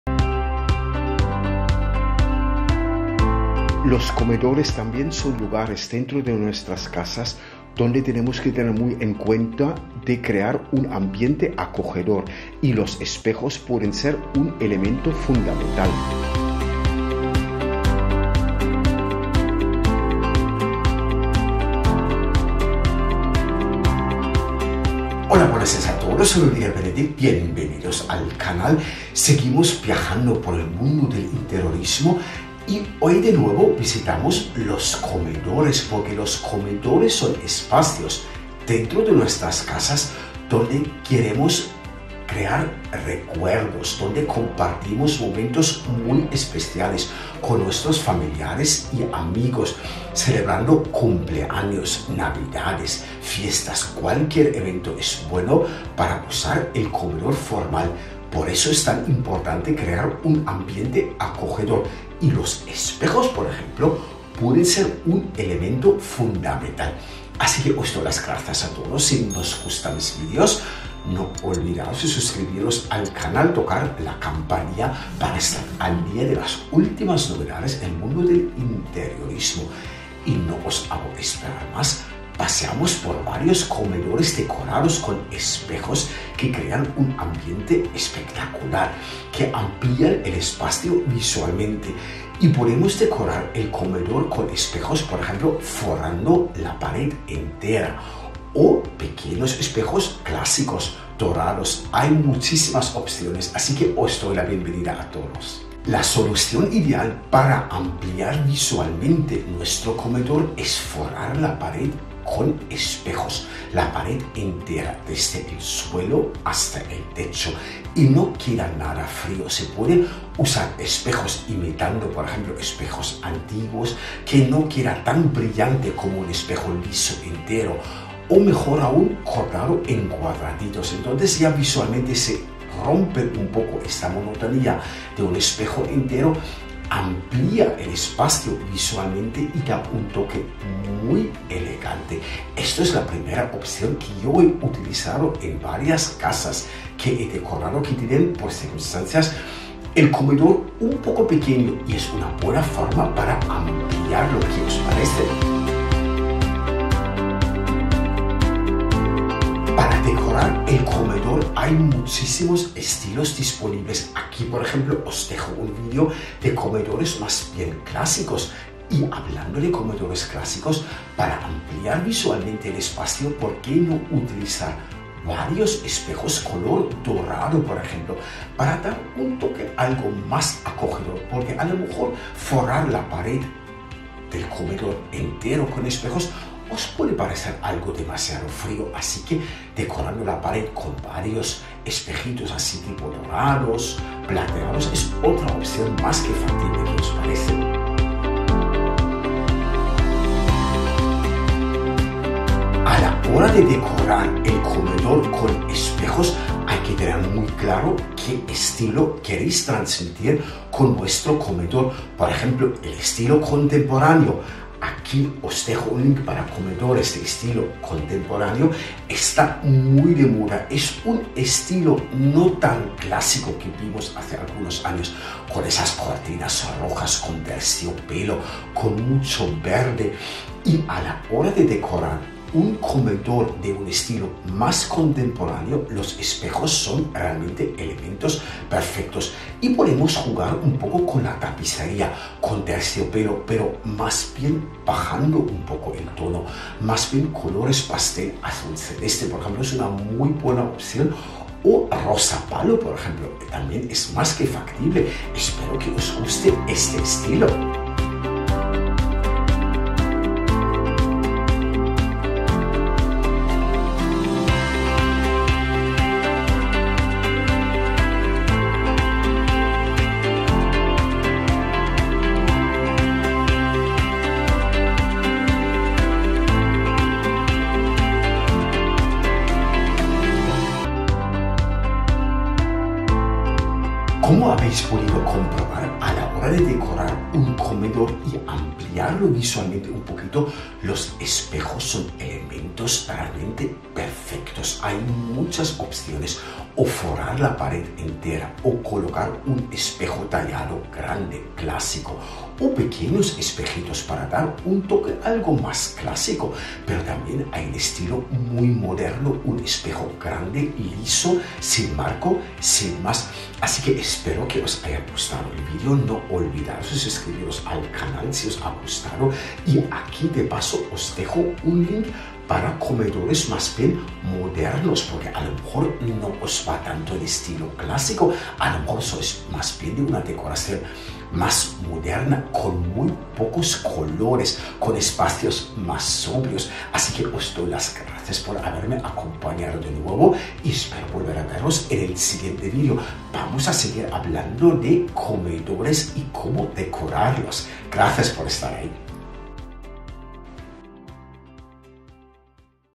Espejos para comedor!🍴✅
👈 Entra ahora ✅ Para dar amplitud a un comedor contemporáneo, no hay mejor elemento que los espejos de comedor. Hubo ocasiones en los cuales, a la hora de decorar una casa, me he encontrado con el hecho de que el comedor no tenía las dimensiones deseadas, pero a la vez se tenía que transmitir amplitud a este espacio tan familiar.

Partes del vídeo:
00:00 Introducción
01:19 Comedores decorados con espejos
01:45 Espejos enteros
02:39 Espejos con color dorado
03:22 Espejos estilo contemporáneo
04:43 Espejos estilo moderno

Para dar una sensación de estar en un comedor amplio, acogedor y elegante a la vez, la mejor opción es recorrer a la magia de los espejos de comedor. Las posibilidades son infinitas, se puede decorar el comedor con espejos tallados, o por ejemplo forrar la pared entera con espejos para comedor modernos.

Cuando se combina con mobiliario ligero, tipo tapicería con tonos pastel, el comedor se convierte en un espacio muy cálido. Y es lo más importante, convertir nuestra casa con su comedor en un hogar acogedor, para que nuestros invitados estén a gusto.